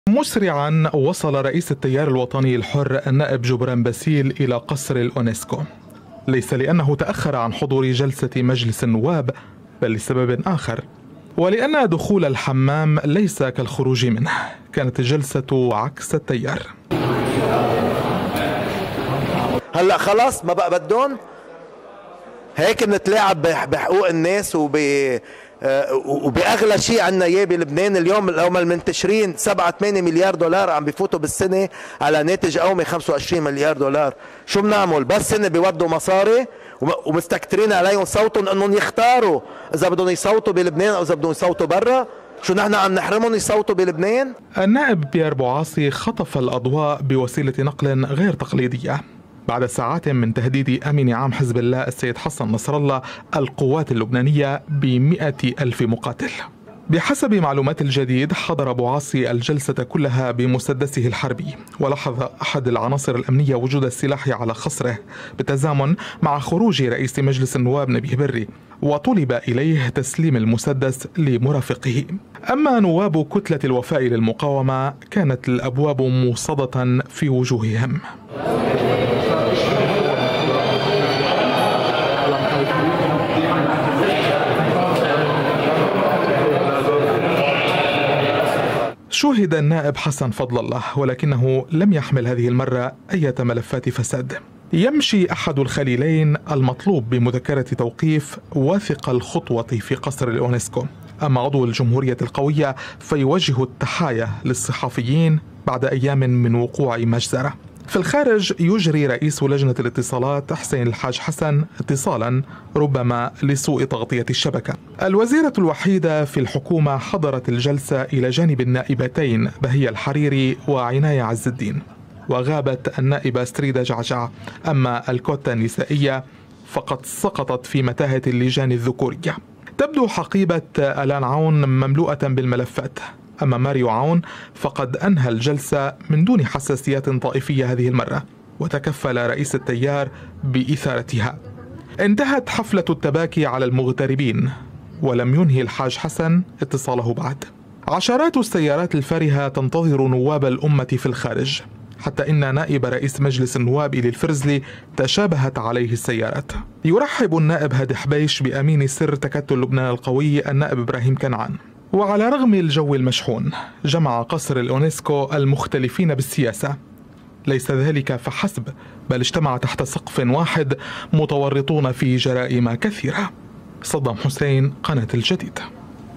مطلوبون في مجلس النواب.. وبيار بو عاصي يحضر بوسيلة نقل غير تقليدية ومسدس
مسرعا وصل رئيس التيار الوطني الحر النائب جبران باسيل إلى قصر الأونيسكو. ليس لأنه تأخر عن حضور جلسة مجلس النواب، بل لسبب آخر..
ولأن دخول الحمام ليس كالخروج منه، كانت الجلسة عكس التيار.
النائب بيار بو عاصي خطف الأضواء بوسيلة نقل غير تقليدية..